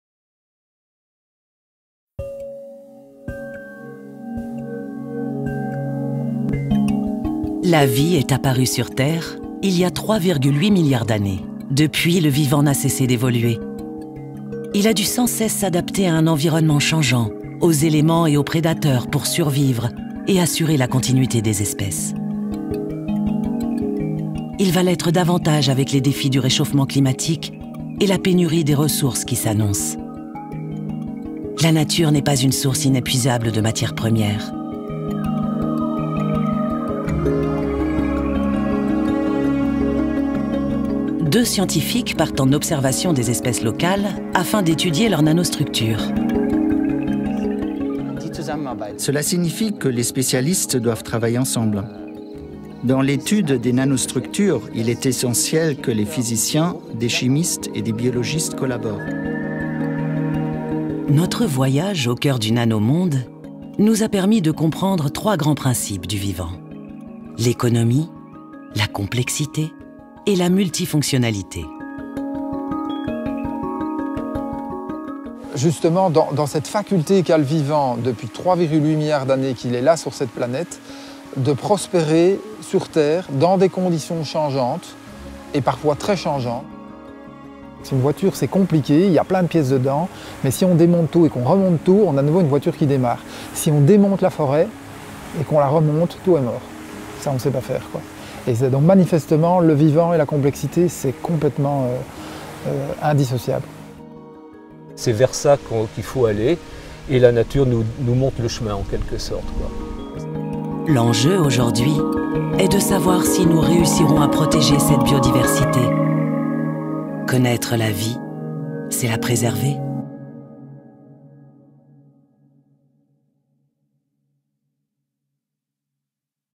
LES NANOSURVIES, LES POUVOIRS INVISIBLES DE LA NATURE.
Un exploration au coeur du nanomonde et des pouvoirs de l'invisible avec le nouveau film de La Belle Socièté Production. A l'éré des nanotechnologies, ce documentaire propose un voyage dans l'infiniment petit sous le regard des scientifiques Serge Berthier et Bernd Schörlhon.
Mieux comprendre la nature pour en protéger la biodiversité, tel est l'enjeu pour notre futur!